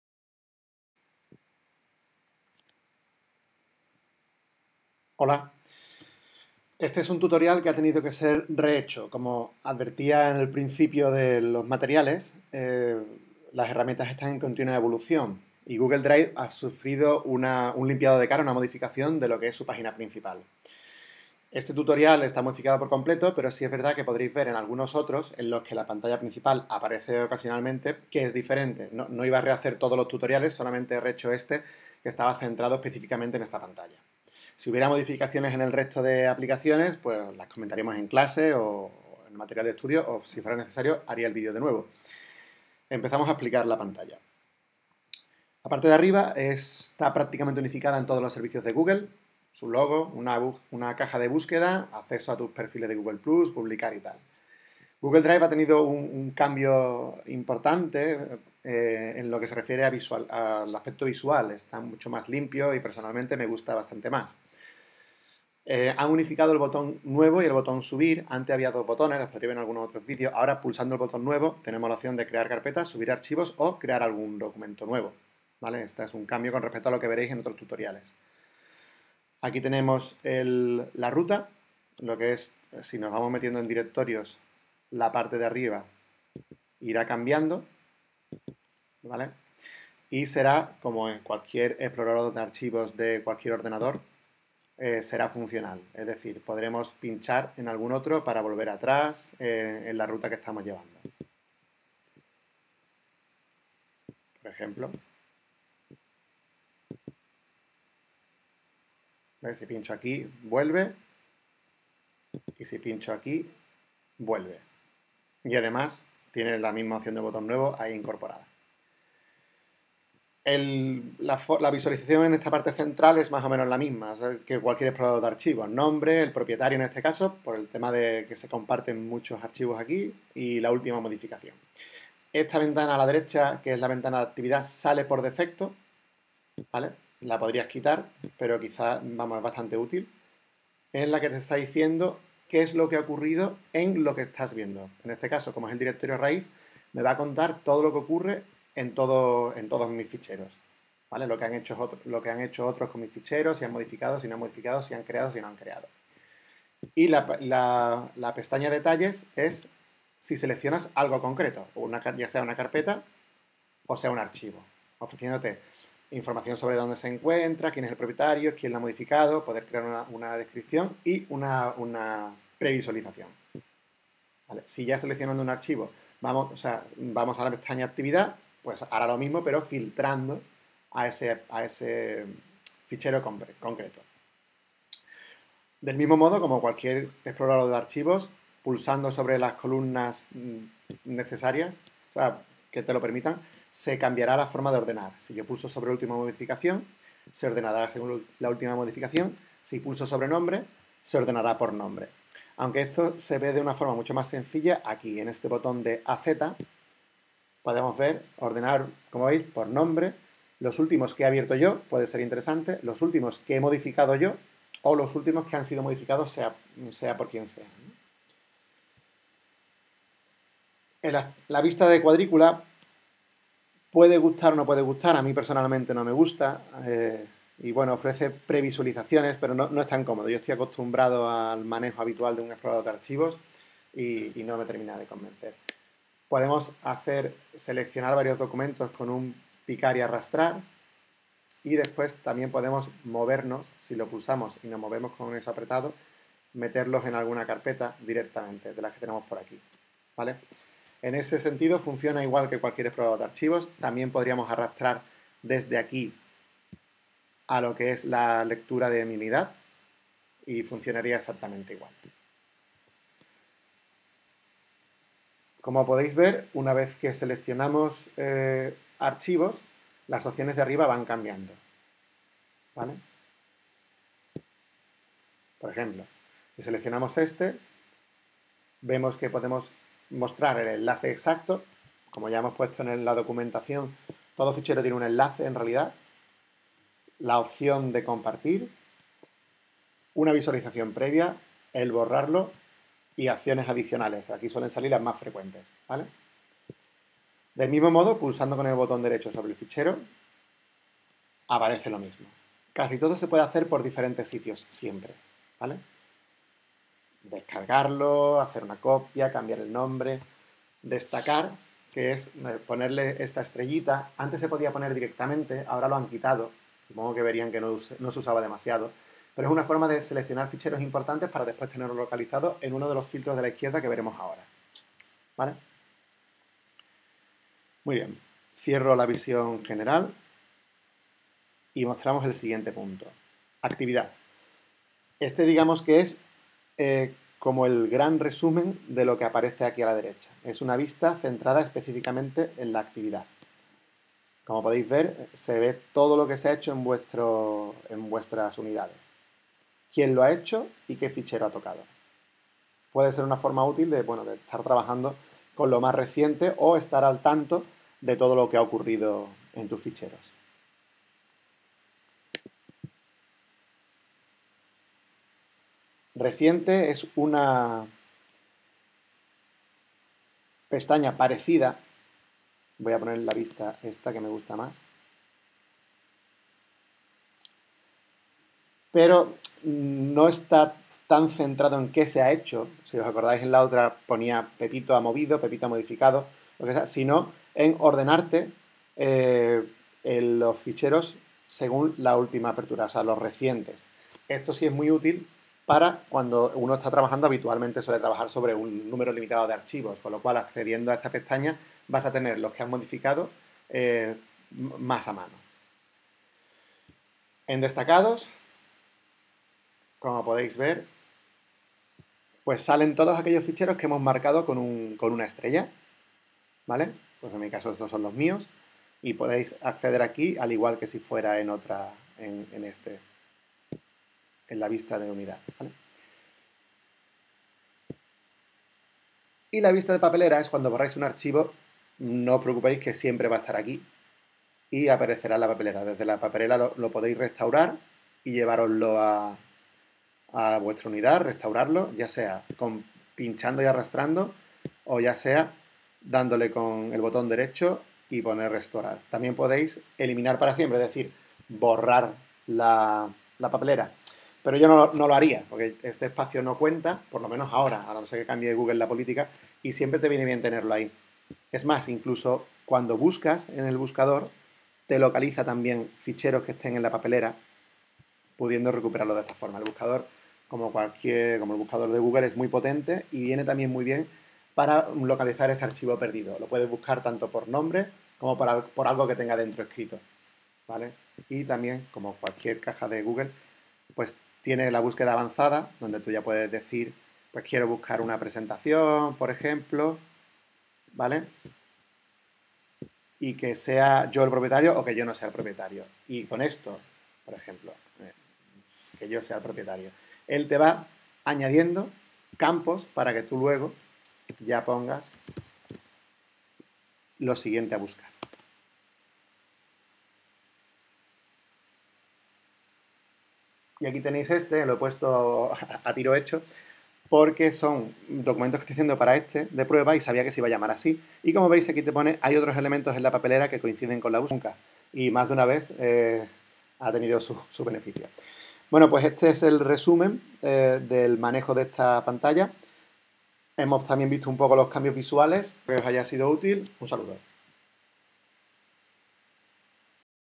Manejo básico de Google Drive
Este vídeo es parte de un curso para ESIC Business & Marketing School.

Mostramos el manejo básico de Google Drive como gestor de archivos. Las diferentes aplicaciones de esta suite se explicarán en vídeos posteriores.

Este vídeo se actualizó tras el cambio en el interfaz de Google Drive acontecido en julio.